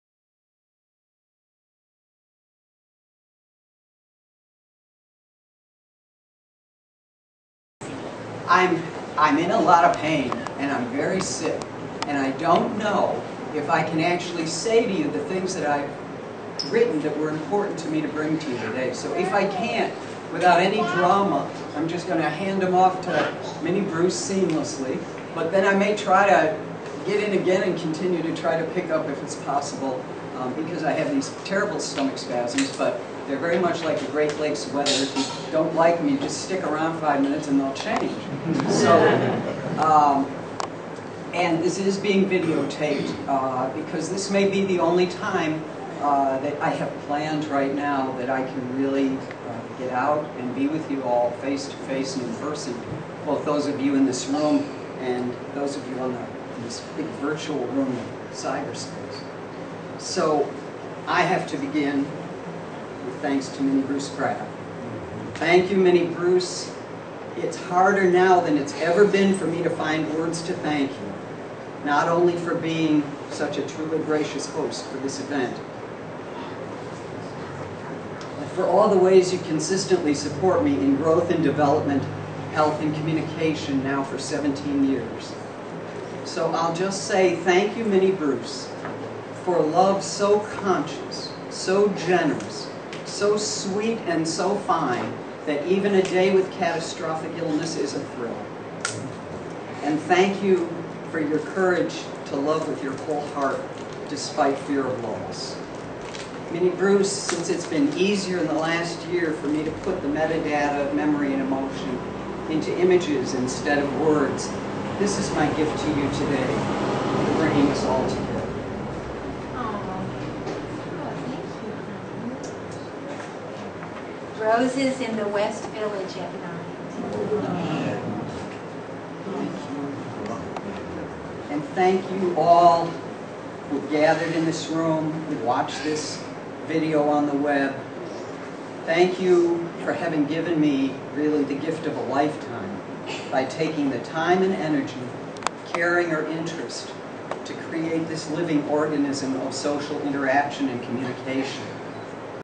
60th Birthday Talk: Part 1: Opening Thanks
This is one of 6 segments of a talk by Leslie Feinberg given on September 5th in Syracuse, NY at her birthday celebration. The celebration included the first showing of many of Leslie's photographs and the publishing of her latest book, "Rainbow Solidarity in Defense of Cuba.
The talk will be available without interruptions on workers.tv in the near future.